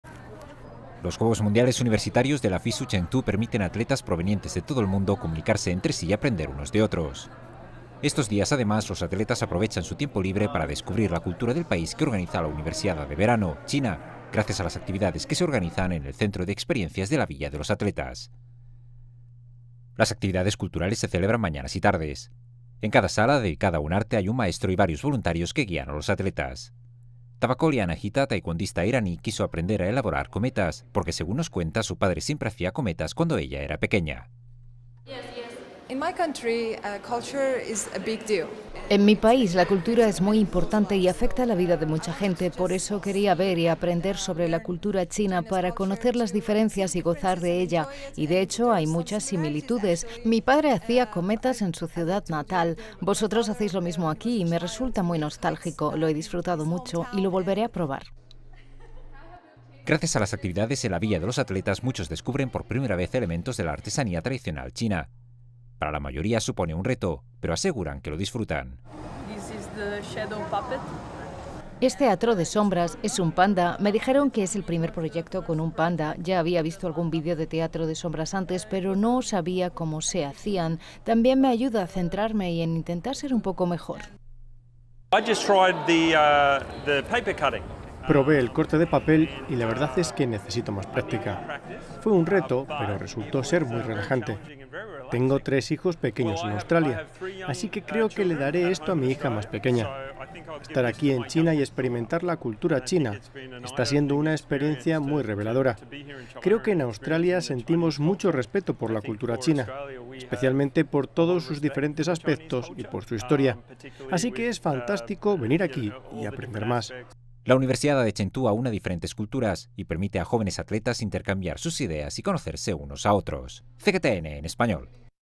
La Villa de los Atletas organiza actividades para que jóvenes atletas conozcan la cultura china
La Villa de los Atletas, donde se hospedan los jóvenes atletas que participan en los Juegos Mundiales Universitarios de la FISU Chengdu, organiza toda una serie de actividades destinadas a dar a conocer elementos de la cultura china. De esta forma, jóvenes de todo el mundo pueden descubrir y practicar artes tradicionales como el teatro de sombras, la confección de cometas o el recorte de papel.